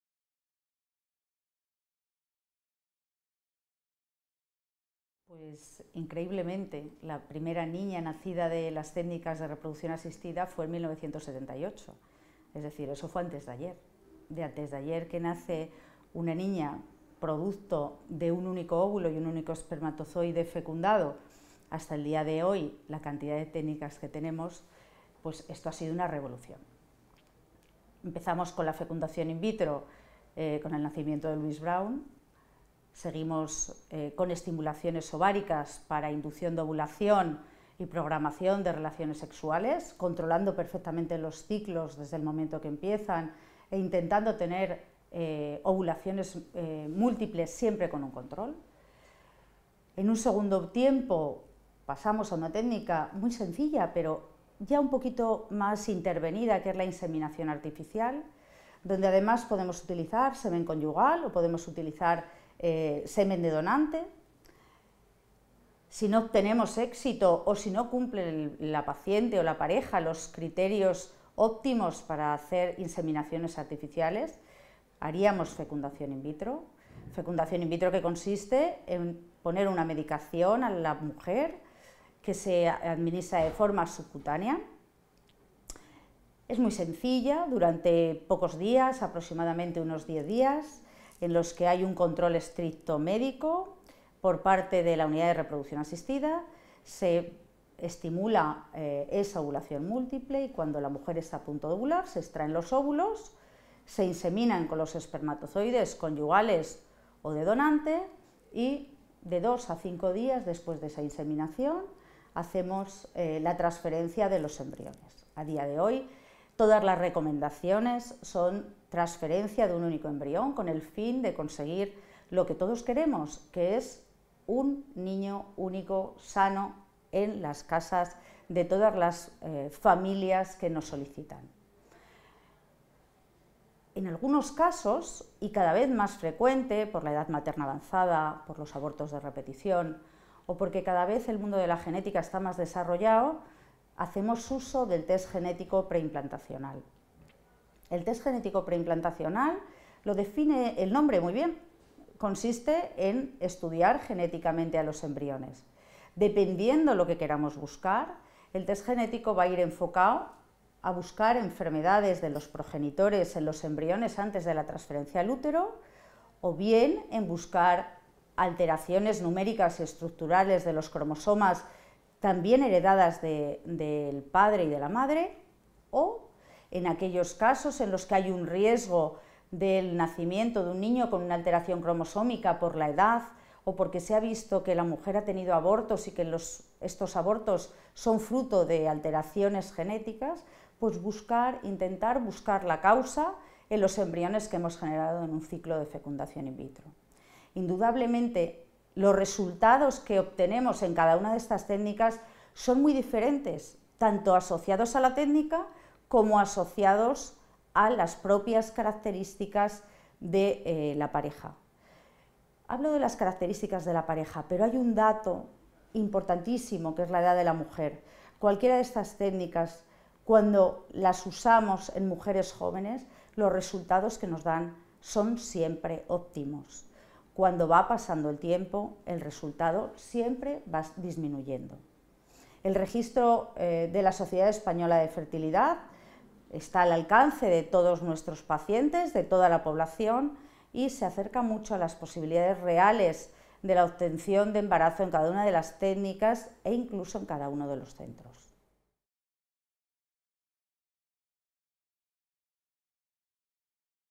Principales Técnicas y resultados en Reproducción Asistida
41 años después de la llegada al mundo de la primera niña nacida por técnicas de reproducción asistida, las opciones que ofrece este servicio de la Fundación Jiménez Díaz, liderado por la Dra. Corazón Hernández, que incluyen desde la estimulación ovárica y la inseminación artificial, a la fecundación in vitro, o la donación de gametos, pasando por el test genético preimplantacional, multiplican las opciones de éxito en el deseo de las parejas que visitan este departamento: tener un hijo sano en casa.